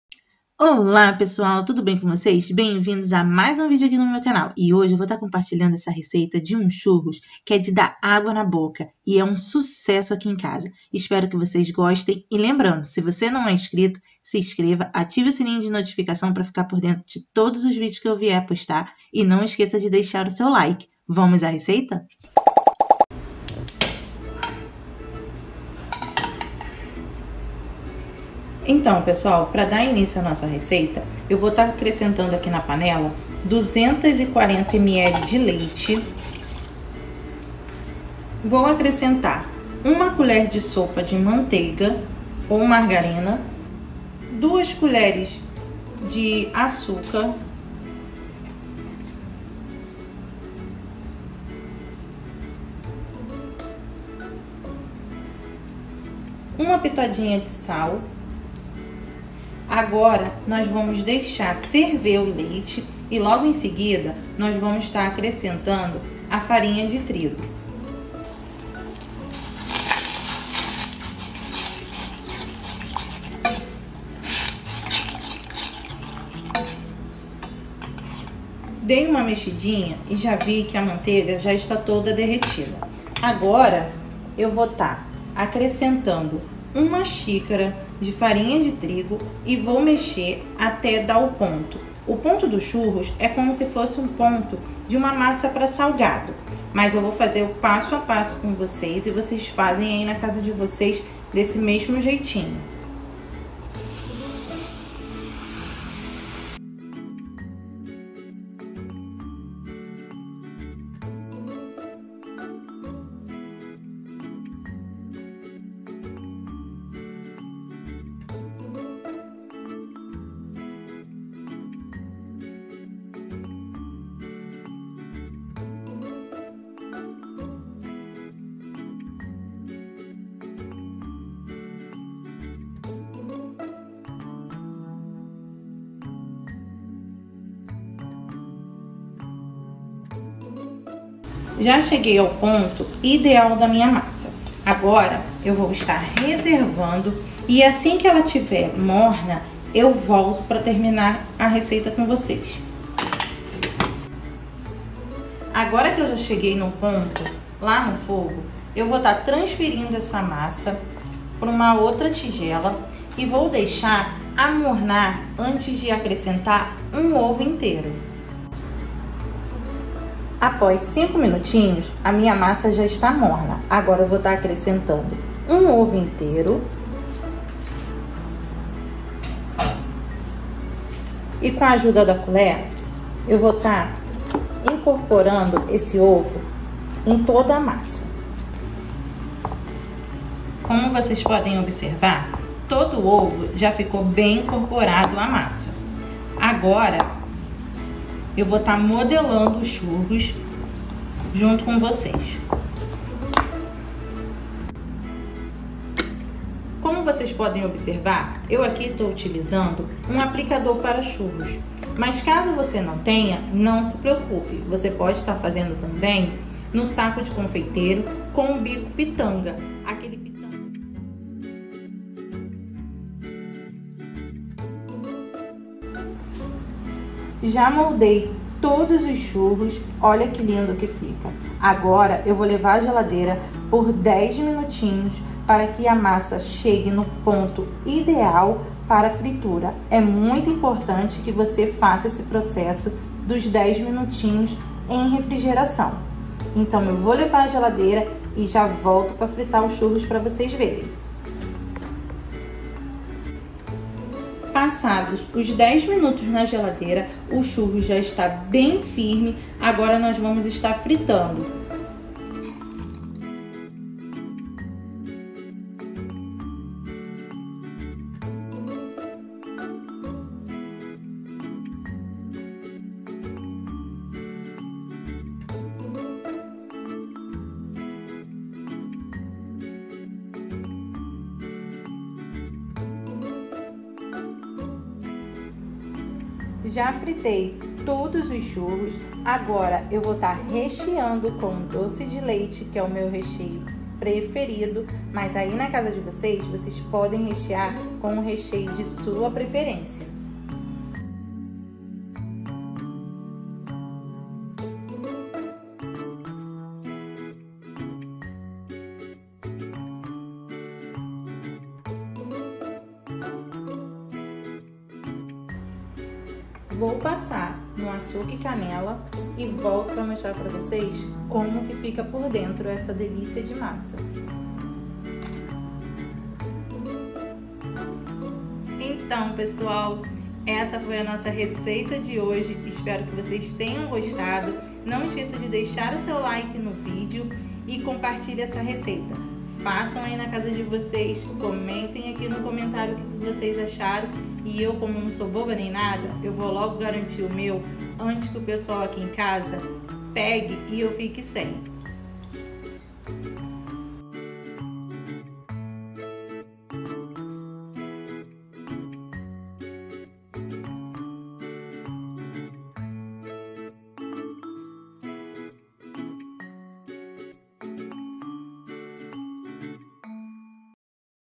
CHURROS Fácil I RECEITA PRÁTICA
Como fazer churros caseiro-simples e delicioso

A receita de hoje, é de CHURROS CASEIRO, com uma receita super facil e que fica uma delícia.
Então...segue a receita!

Todos os ingredientes utilizados para receita está no passo a passo do vídeo .

As crianças vão adorar e pode rechear com o sabor de sua preferência